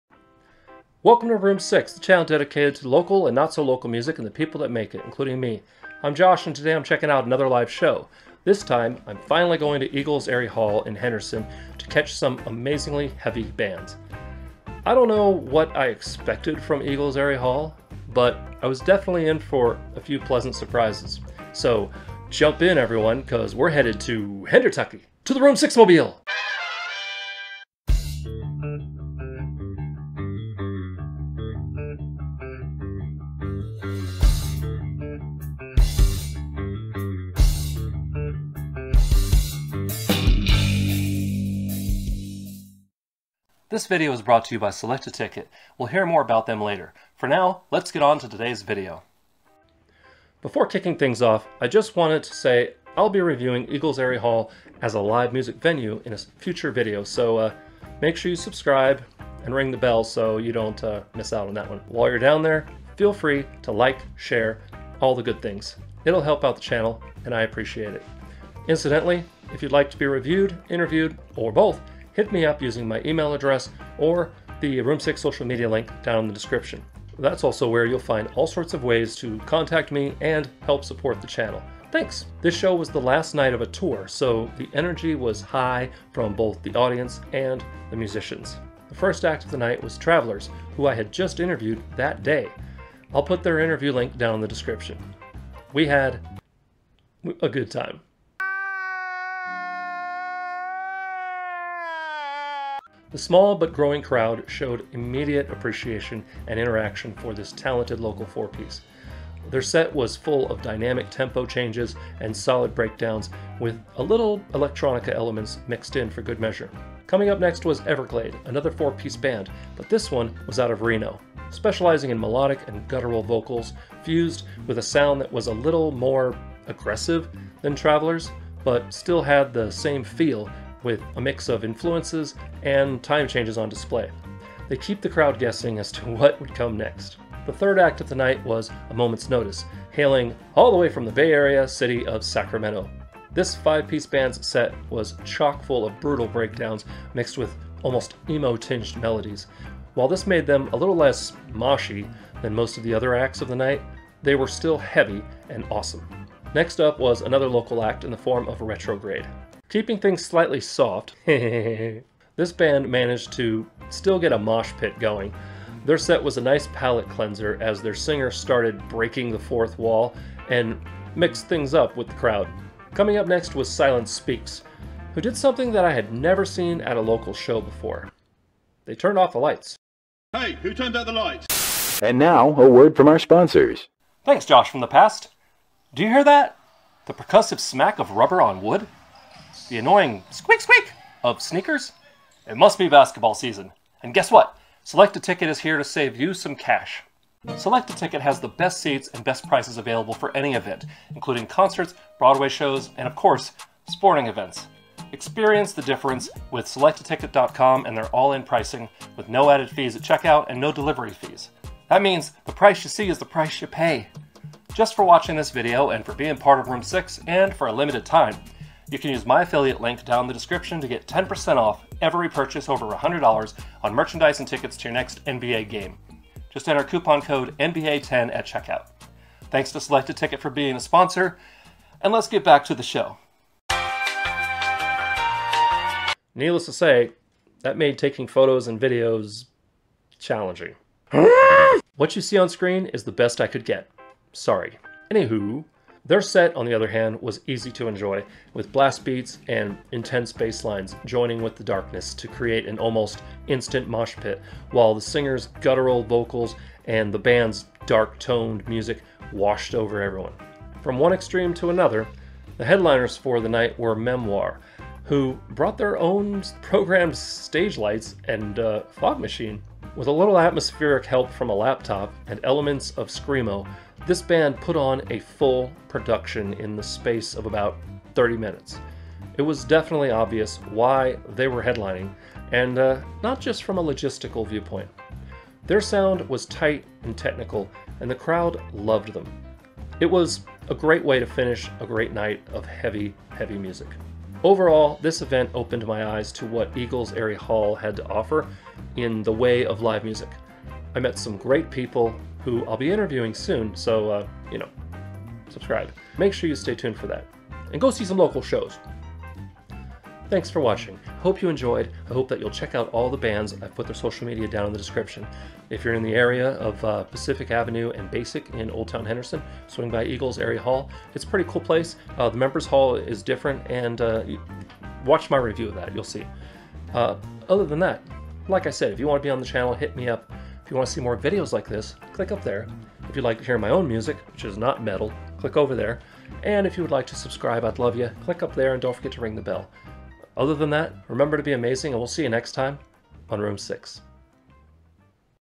ROOM 6 REVIEWS #180 - Memoire & Friends @ Eagles Aerie Hall! [LIVE SHOW REVIEW]
Today I'm reviewing my 1st live show at Eagles Aerie Hall featuring MEMOIRE, SILENCE SPEAKS, RETROGRVDE, EVERGLADE, A MOMENT'S NOTICE, & TRVLRS. Enjoy! ;-D

FEATURED ACTS:
MEMOIRE - @memoirenv
SILENCE SPEAKS - @silencespeakslv
RETROGRVDE - @retrogrvdeband
EVERGLADE - @everglade_nv
A MOMENT'S NOTICE - @amomentsnoticeofficial
TRVLRS - @trvlrsband